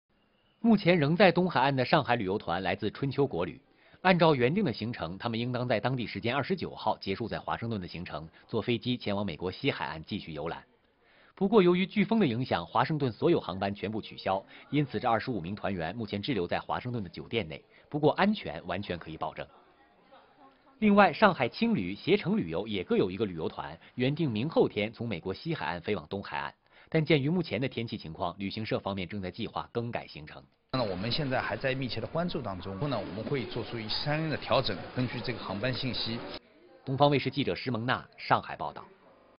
25名上海游客因飓风桑迪影响滞留美国华盛顿 目前安全
据上海市旅游局10月30日透露，目前上海共有14个旅游团、共计172名游客在美国，不过，这其中，受到飓风影响的只有一个团，共25名游客。